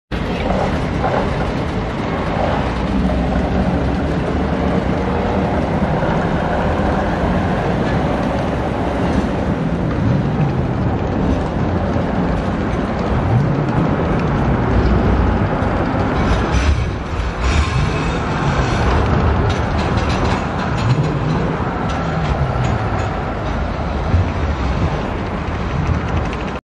Авариен автобус Чавдар 11г5 влиза в депо Клокотница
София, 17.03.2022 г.